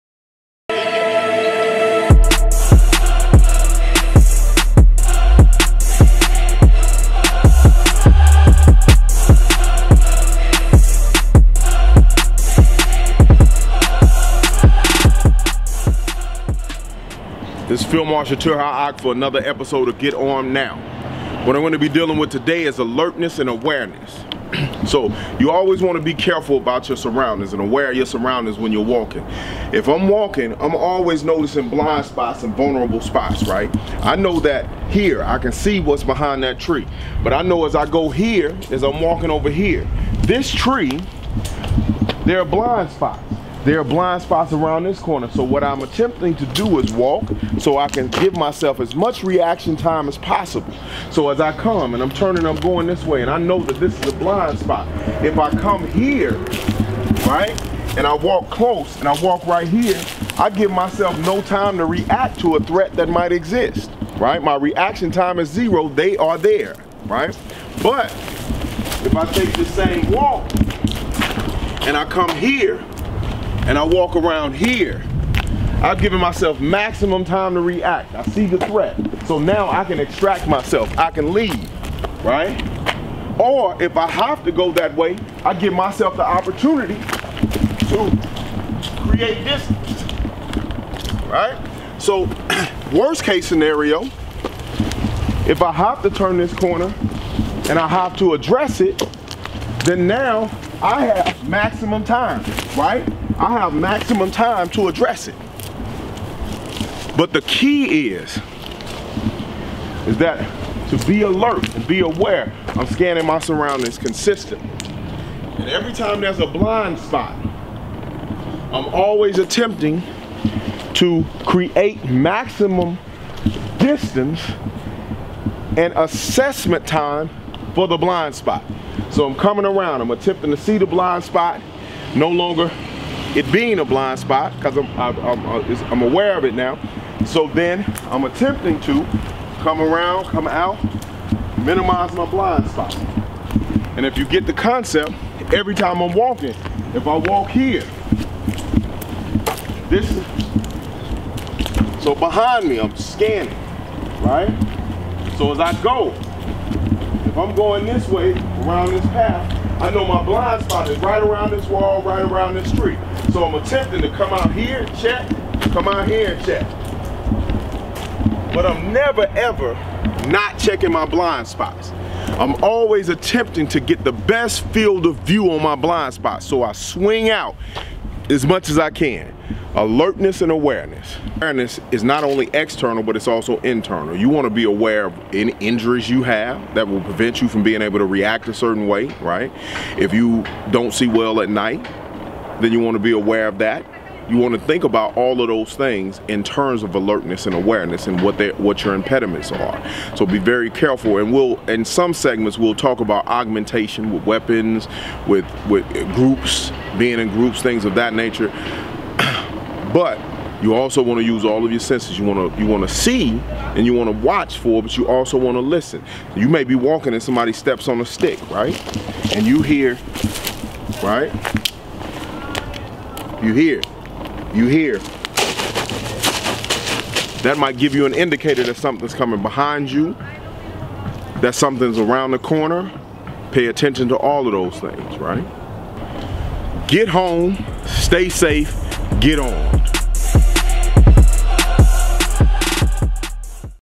GET A.R.M.E.D. NOW "ALERTNESS & AWARENESS"
In this episode we Field Marshall Tur-Ha Ak talks about the importance of awareness.